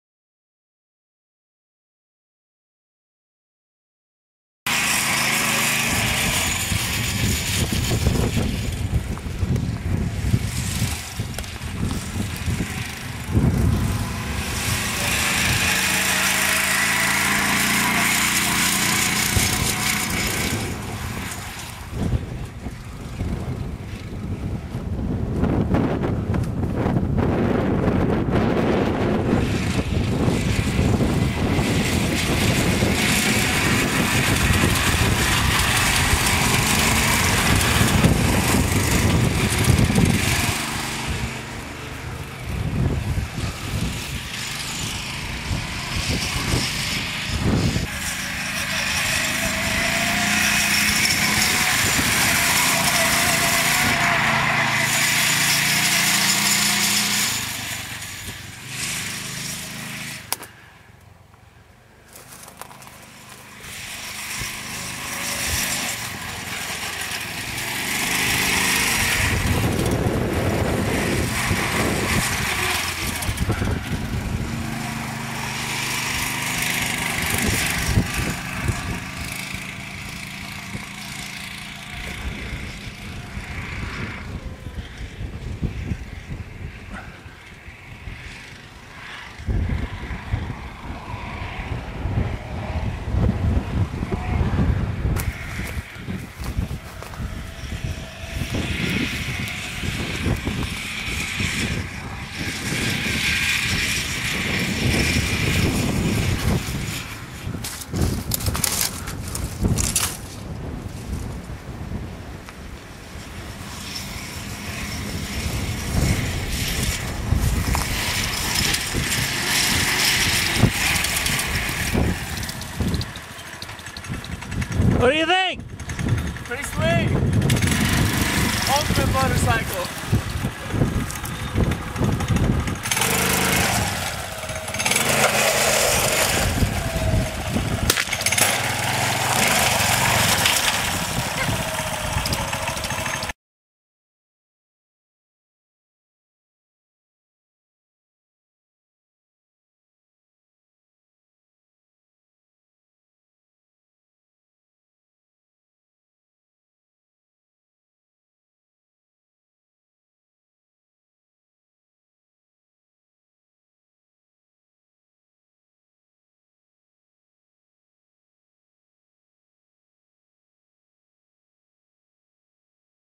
Rokon Trail Breaker in Western Kansas
Christian riding the Rokon